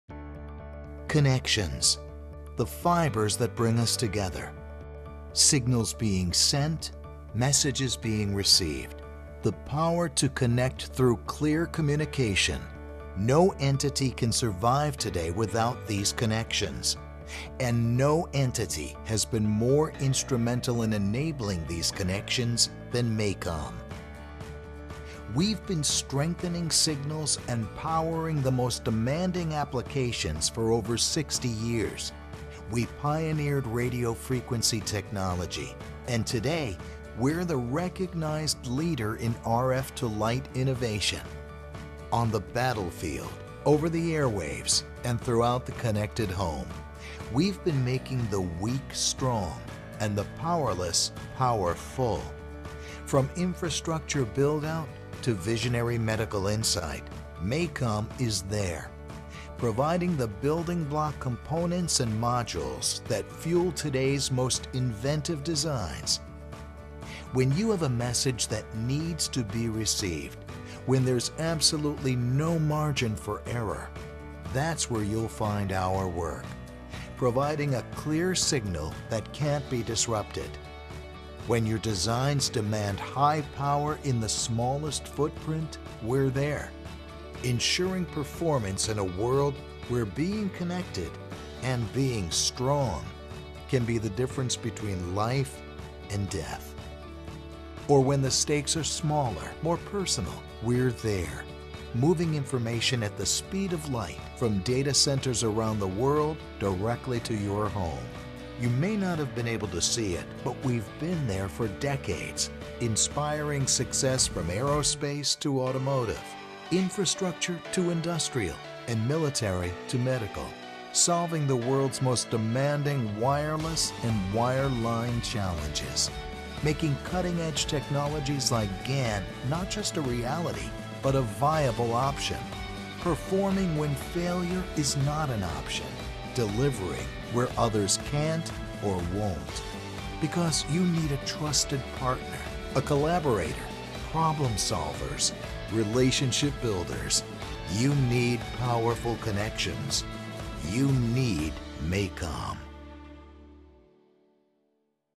MACOM Overview
MACOM designs and manufactures high-performance semiconductor products for the Telecommunications, Industrial and Defense and Datacenter industries.  MACOM services over 6,000 customers annually with a broad product portfolio that incorporates RF, Microwave, Analog and Mixed Signal and Optical semiconductor technologies. MACOM has achieved certification to the IATF16949 automotive standard, the ISO9001 international quality standard and the ISO14001 environmental management standard.  MACOM operates facilities across the United States, Europe, Asia and is headquartered in Lowell, Massachusetts.